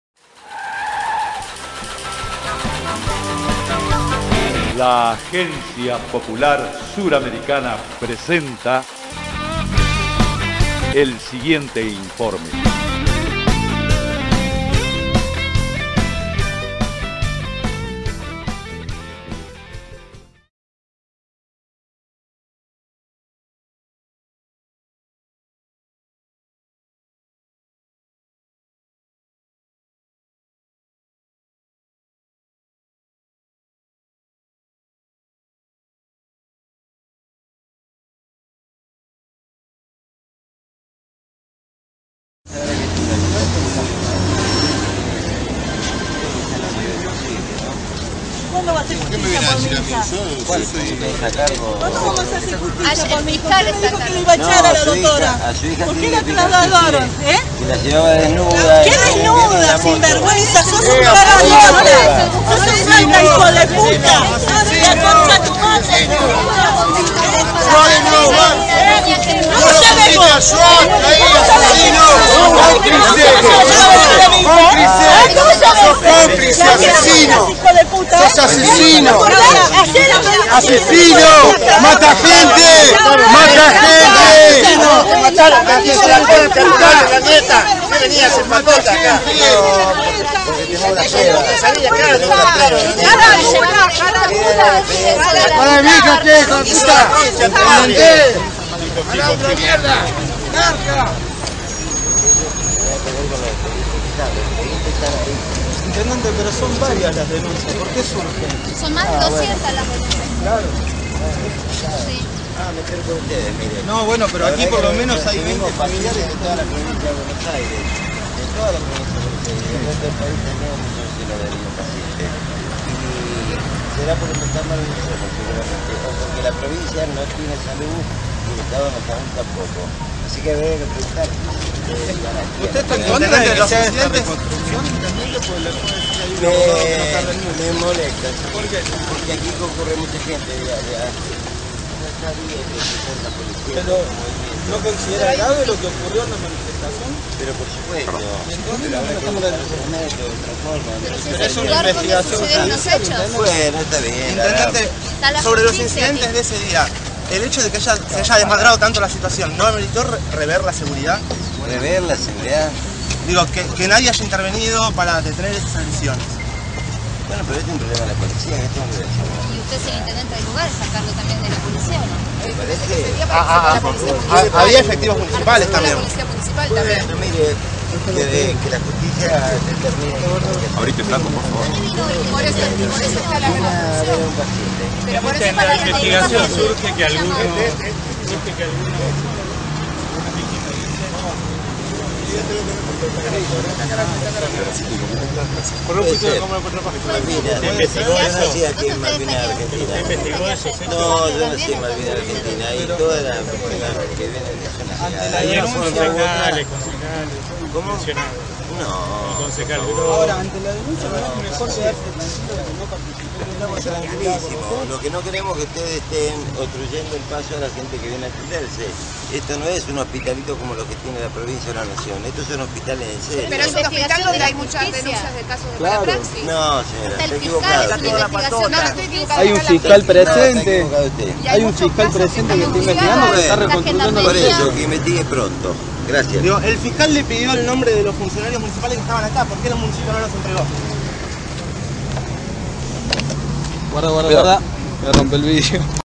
Jesus Cariglino y la doble moral ante la muerte de un niño Qon
APCS. El Intendente Jesus Cariglino y la doble moral ante la muerte de un niño Qon. Por medio de un comunicado en Facebook, el intendente fustigó al Jefe de Gabinete Jorge Milton Capitanich por la muerte de un niño Qon, pero el tuvo una peor actitud cuando minimizó la muerte de Melani y cargó toda la responsabilidad en su madre, Claudia Ledesma, (Ver nota completa